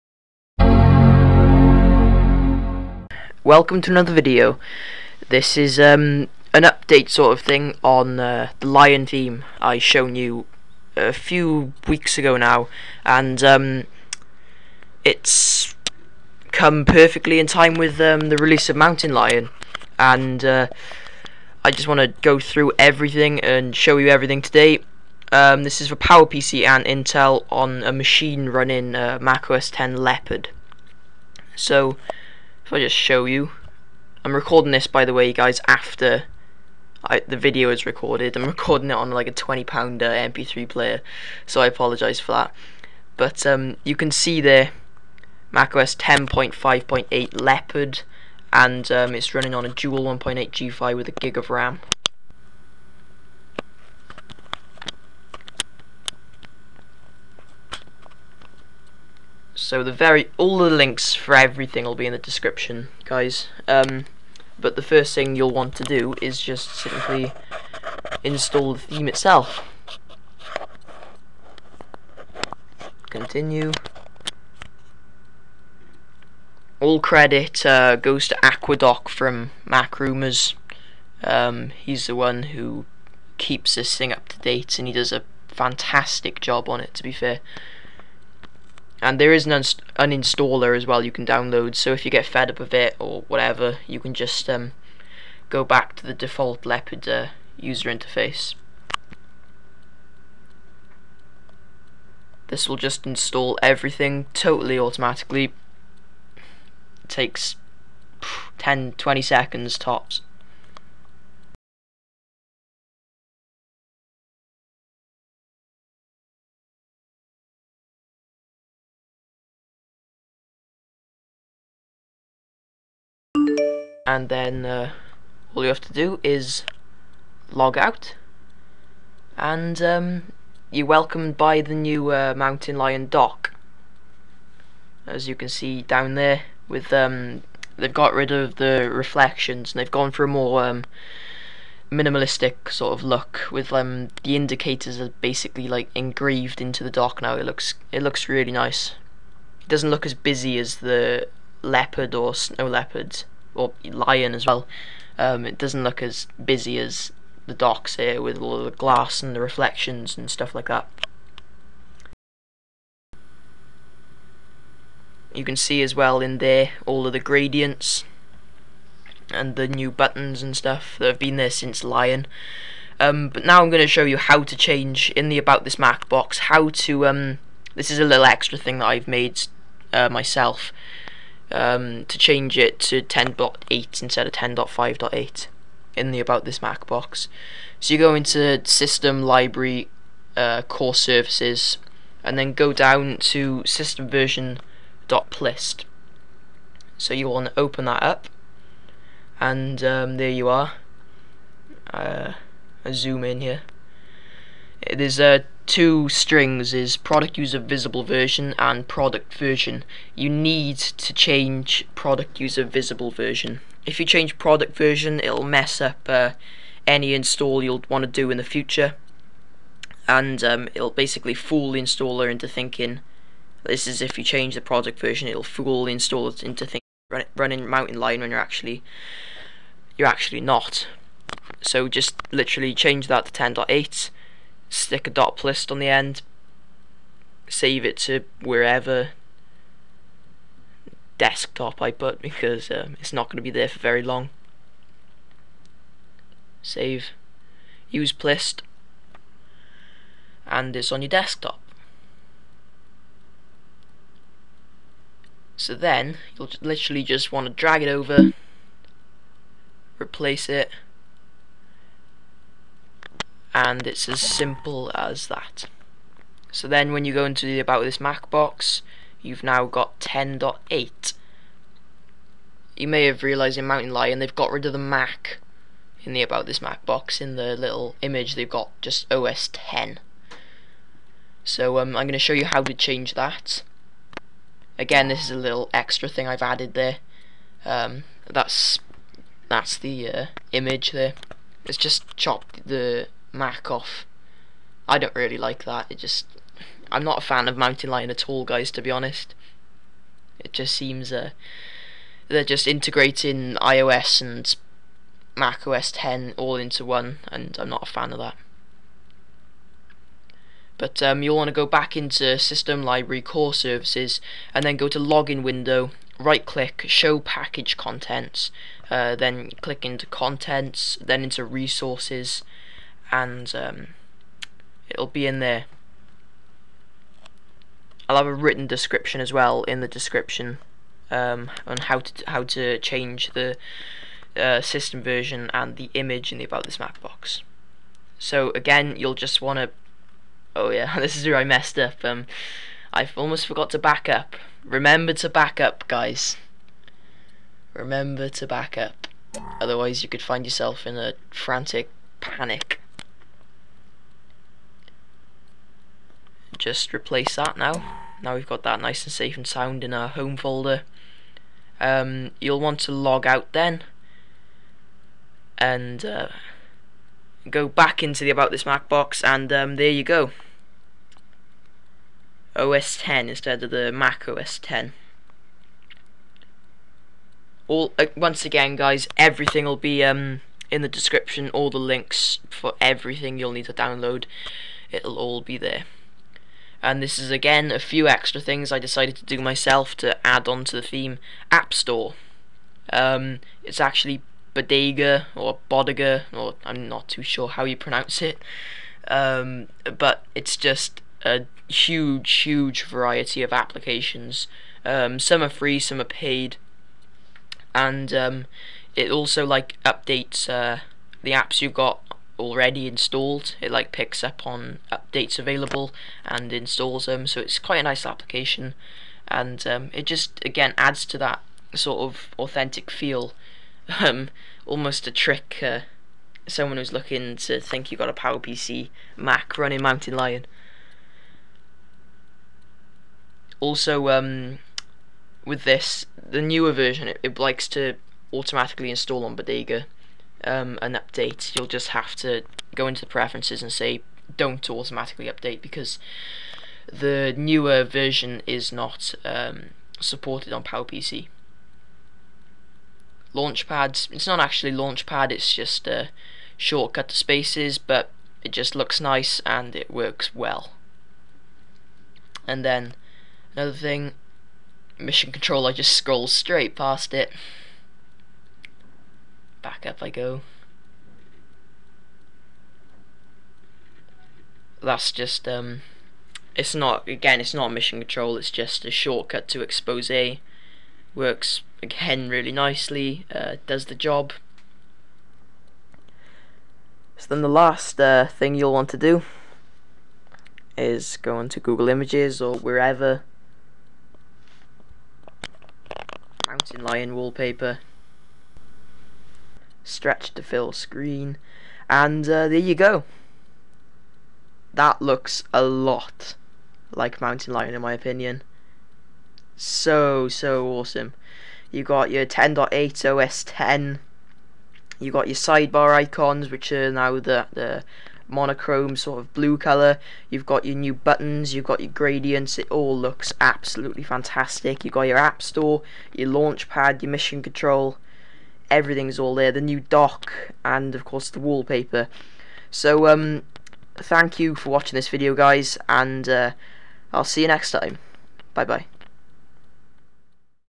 OS X 10.8 Mountain Lion Theme for OS X 10.5 Leopard | The PowerPC Hub
*Written Help Below*

UPDATED JULY 2013 - V3.0 has been released! Links updated.

Like the new look in Mountain Lion? Have a PowerPC Mac? Look no further! I go through everything step by step on how to take your PowerPC machine running Leopard into the wonderful funland of Mountain Lion! Enjoy the video :)

Bodega 1.5.3 (Had To Host Myself Because 1.5.3 Isn't Available Anymore)

1. You'll need to replace the current MacOSX.tif (remember to back up) with the new one at; /System/Library/CoreServices/
2. To change the OS X version to 10.8 go to; /System/Library/CoreServices/SystemVersion.plist

Open the plist file up in textedit and change ProductUserVisibleVersion to 10.8. Then you'll have to save as SystemVersion.plist on your desktop or wherever you can find it (can't just save due to permissions, and remember to add the plist on the end). Then simply drag the new SystemVersion.plist over and replace the old one.